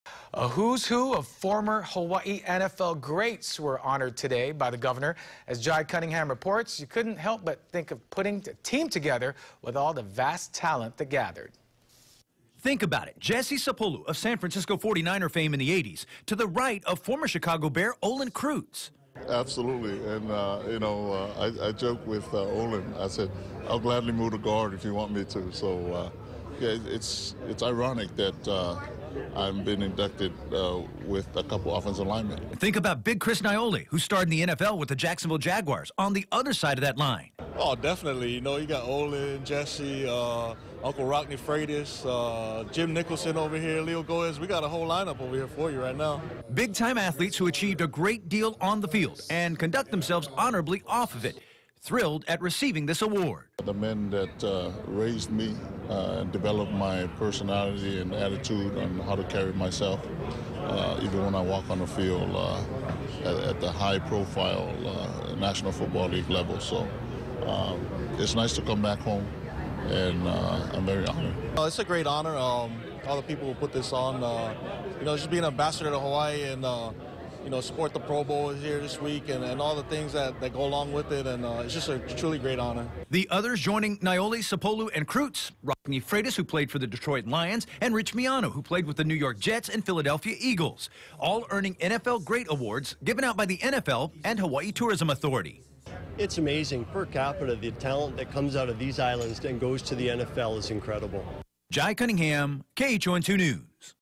Hawaii NFL greats honored at Washington Place
A who's who of former Hawaii NFL greats were honored on Friday by the governor.